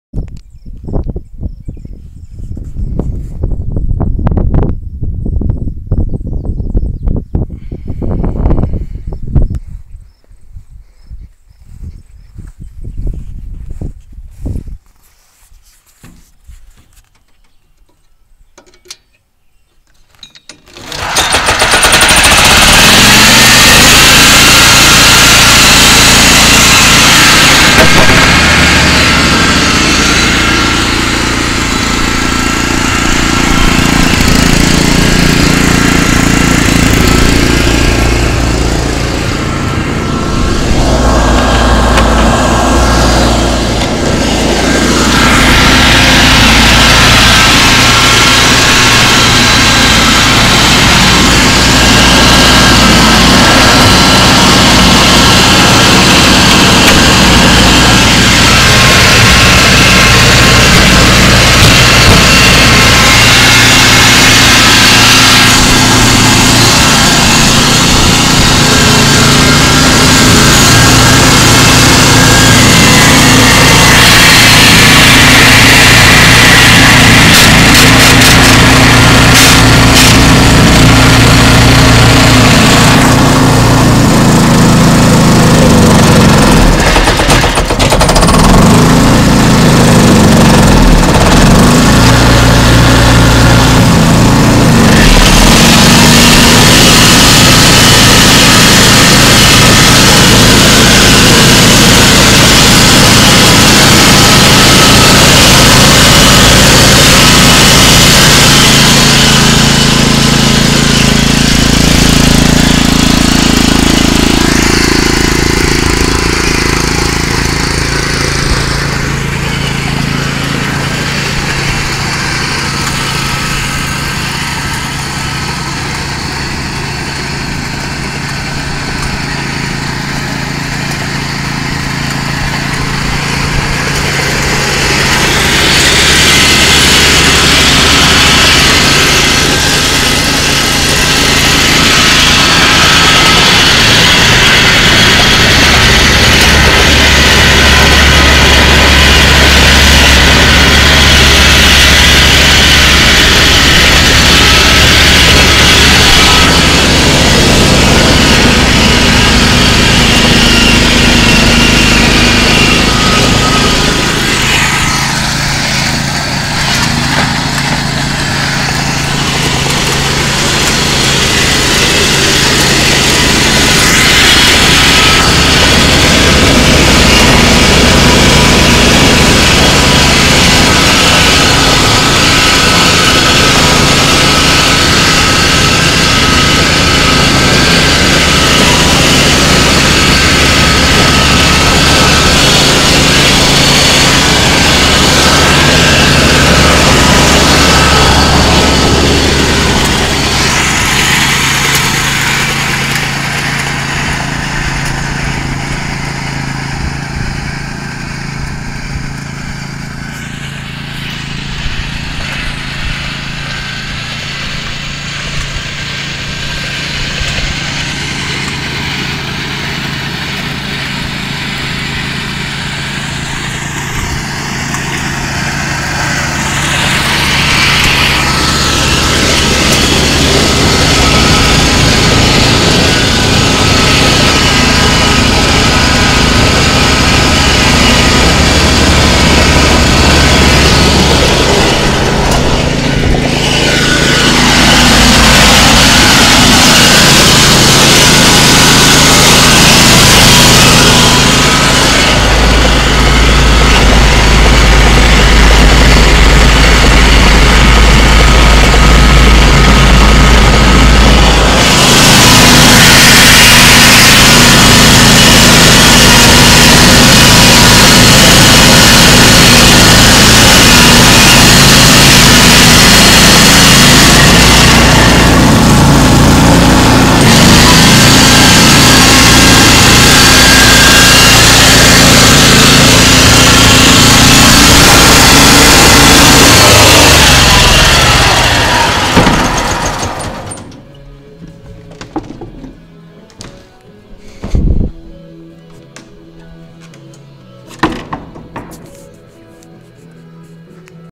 halftrack
A halftrack i made for airsoft